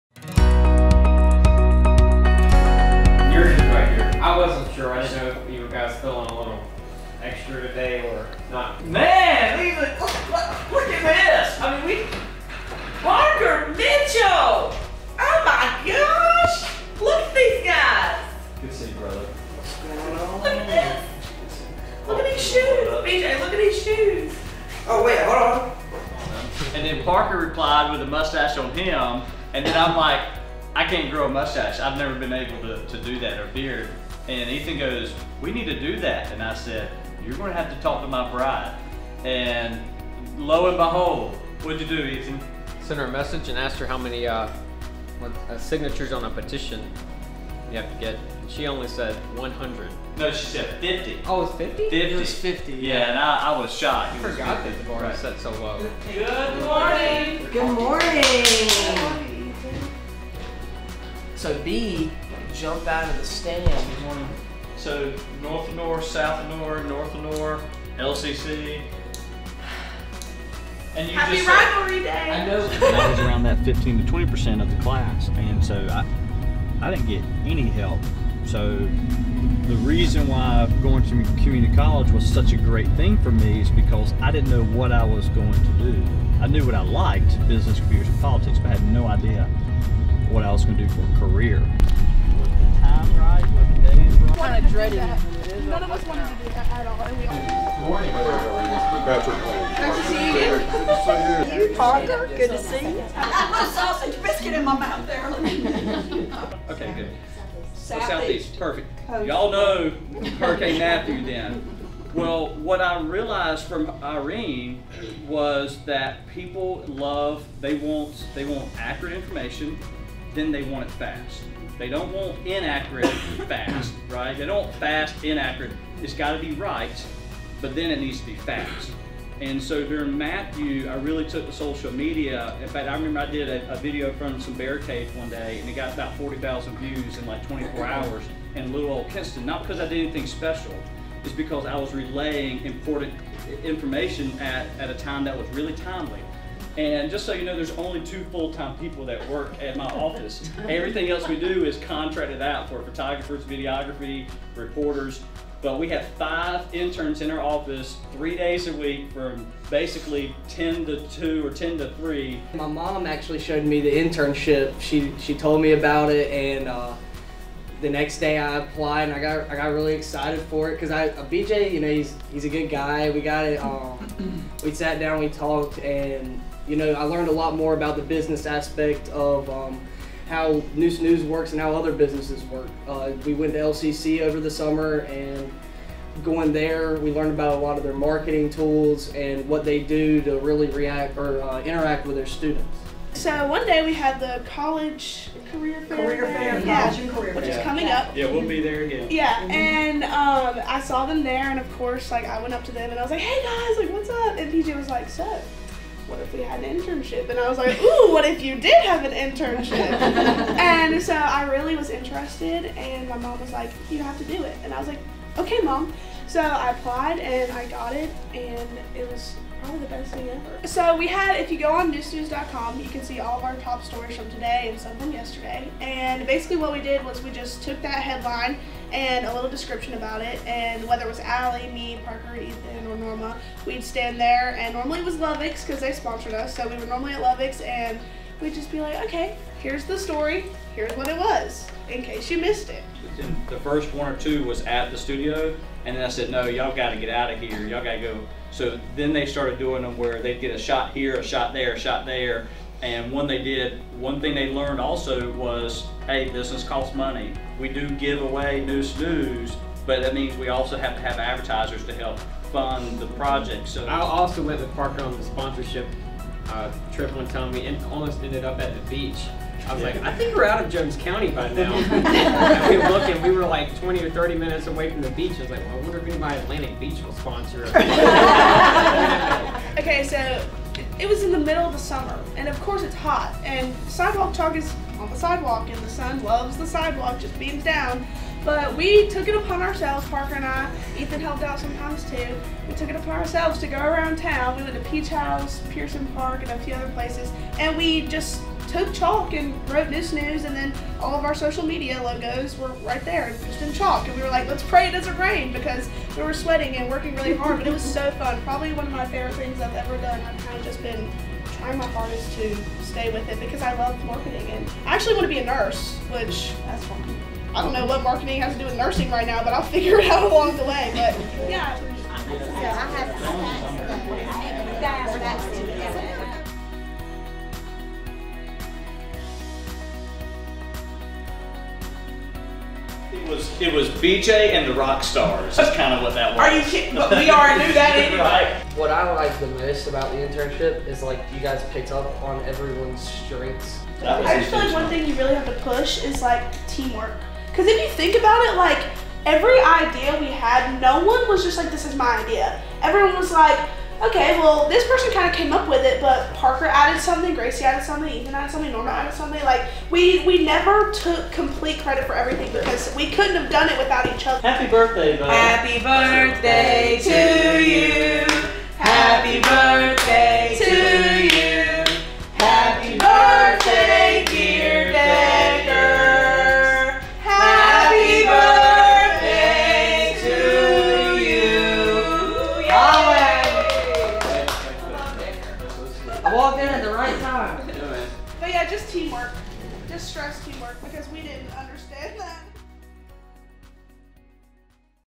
The Journey Episode 19 - 2019 Interns Victory Tour
Our 2019 Summer Interns joined me recently to discuss the integration of their education and professional goals with southeastern NC Career and Technical Education professionals who met at Kinston High School. The inaugural internship experiment at Neuse News and Magic Mile Media was a huge success because we all worked to match their skill sets and talents with projects for our companies and clients.

Three of our five interns (Gracie Herring, Parker Mitchell and Ethan Cahoon) discussed our program with these professionals and then we went back to our office and chatted about improvements for our 2020 Summer Internship Program.